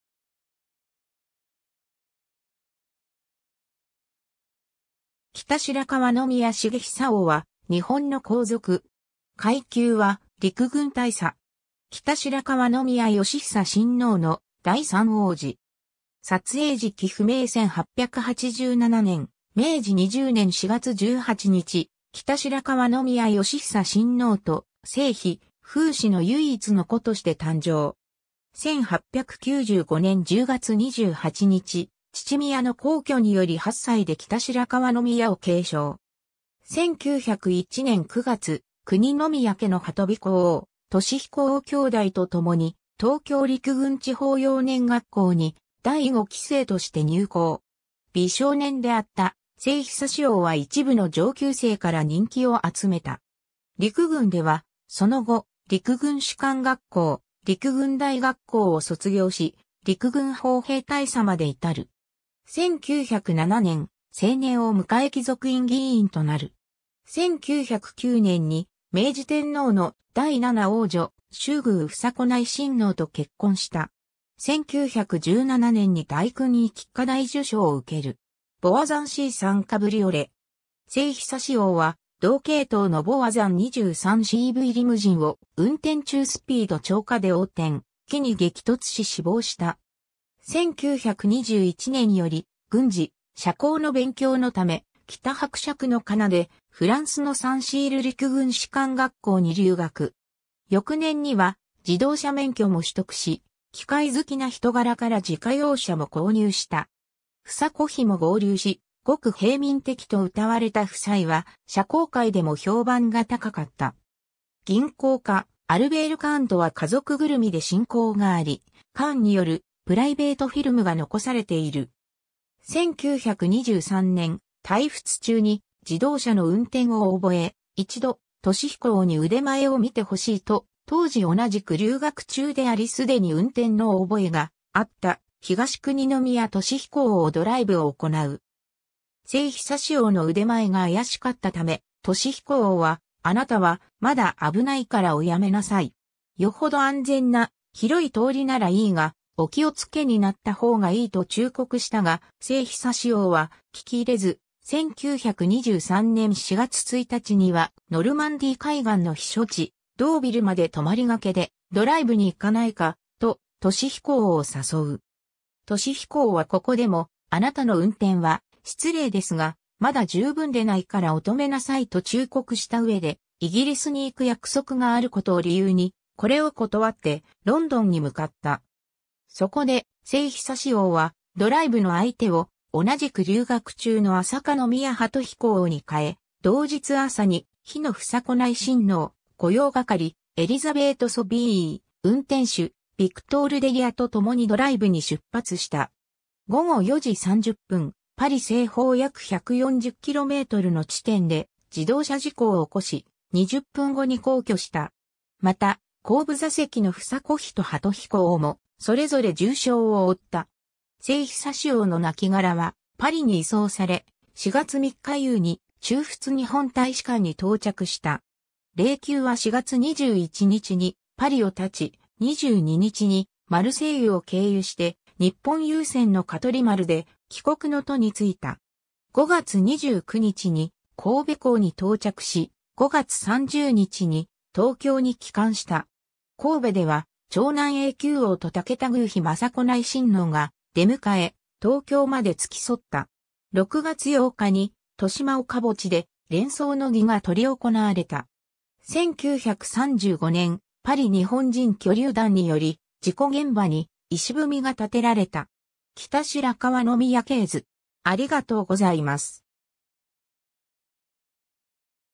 北白川宮成久王
日本の王_(皇族)
北白川宮成久王（きたしらかわのみや なるひさおう、1887年〈明治20年〉4月18日 - 1923年〈大正12年〉4月1日）は、日本の皇族。
階級は陸軍大佐。
北白川宮能久親王の第3王子。
 撮影時期不明 1887年〈明治20年〉4月18日、北白川宮能久親王と正妃富子（伊達宗徳侯爵の次女）の唯一の子として誕生。
1895年（明治28年）10月28日、父宮の薨去により8歳で北白川宮を継承。
 1901年（明治34年）9月、久邇宮家の鳩彦王・稔彦王兄弟とともに、東京陸軍地方幼年学校に第5期生として入校。
美少年であった成久王は一部の上級生から人気を集めた。
陸軍では、その後、陸軍士官学校（20期）、陸軍大学校（27期）を卒業し陸軍砲兵大佐まで至る。
 1907年（明治40年）、成年を迎え貴族院議員となる。
 1909年（明治42年）に明治天皇の第7皇女・周宮房子内親王と結婚した。
1917年（大正6年）に大勲位菊花大綬章を受ける。
 ヴォワザンC3カブリオレ。
成久王は同系統のヴォワザン23CVリムジンを運転中スピード超過で横転、木に激突し死亡した。
 1921年（大正10年）より軍事・社交の勉強のため、「キタ伯爵」の仮名でフランスのサン・シール陸軍士官学校に留学。
 翌年には自動車免許も取得し、機械好きな人柄から自家用車（ヴォワザン23CV、排気量3970cc。）も購入した。
 房子妃も合流し、「ごく平民的」と謳われた夫妻は社交界でも評判が高かった。
銀行家アルベール・カーンとは家族ぐるみで親交があり、カーンによるプライベートフィルムが残されている。
 1923年（大正12年）、滞仏中に自動車の運転を覚え、「一度、稔彦王に腕前を見てほしい」と、当時同じく留学中であり既に運転の覚えがあった東久邇宮稔彦王をドライブを行う。
成久王の腕前が怪しかった為、稔彦王は「あなたはまだ危ないからおやめなさい。よほど安全な広い通りならいいが、お気をつけになった方がいい」と忠告したが、成久王は聞き入れず、1923年4月1日には「ノルマンディー海岸の避暑地ドーヴィルまで泊りがけでドライブに行かないか」と稔彦王を誘う。
稔彦王はここでも「あなたの運転は、失礼ですが、まだ十分でないからお止めなさい」と忠告したうえで、イギリスに行く約束があることを理由にこれを断って、ロンドンに向かった。
 そこで成久王はドライブの相手を同じく留学中の朝香宮鳩彦王に変え、同日朝に妃の房子内親王、御用掛エリザベート・ソビー (Elisabeth Sauvy)、運転手ヴィクトール・デリア (Victor Déliât) と共にドライブに出発した。
 午後4時30分、パリ西方約140ｋｍの地点で自動車事故を起こし、20分後に薨去した。
また、後部座席の房子妃と鳩彦王も、それぞれ重傷を負った。
 成久王の亡骸はパリに移送され、4月3日夕に駐仏日本大使館に到着した。
霊柩は4月21日にパリを発ち、22日にマルセイユを経由して日本郵船の香取丸で帰国の途についた。
 5月29日に神戸港に到着し、5月30日に東京に帰還した。
神戸では長男永久王(当時13歳)と竹田宮妃昌子内親王が出迎え、東京まで付き添った。
6月8日に豊島岡墓地で斂葬の儀が執り行われた。
 1935年（昭和10年）、パリ日本人居留団により事故現場に碑が建てられた。
 北白川宮系図